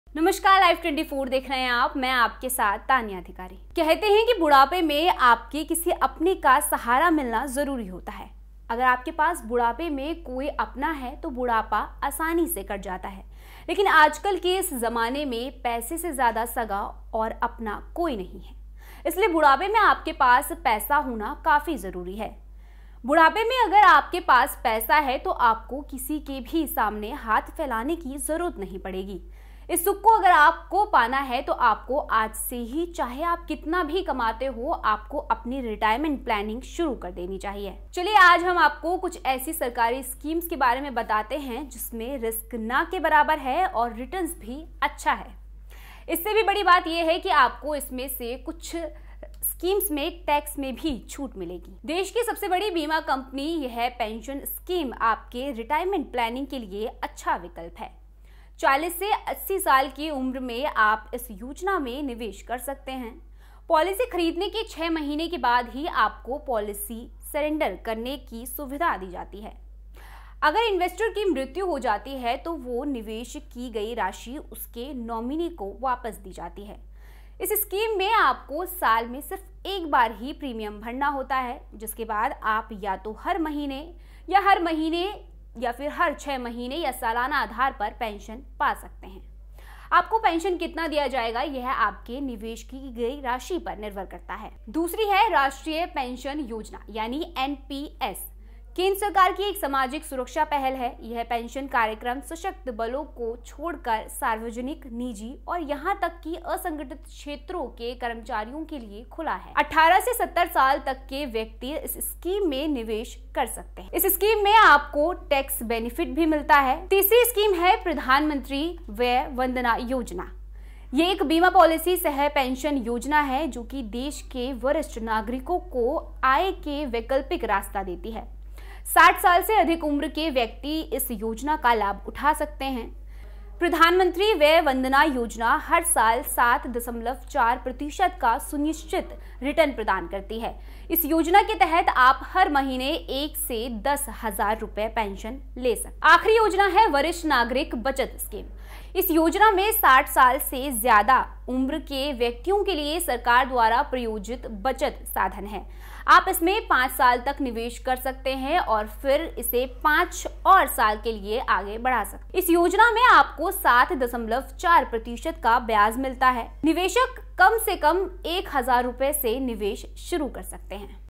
Senior Citizens के लिए इन योजनाओं में निवेश करना है बेहतरीन विकल्प | yojana
In old age, it is important to have support from someone close to you. If you have a loved one in old age, then old age gets passed easily. But in this day and age, no one is more important than money, hence it is very important for you to have money in old age.
If you have money in old age, you will not need to lend your hand to anyone. If you want to achieve this happiness then you should start your retirement planning from today itself, no matter how much you earn.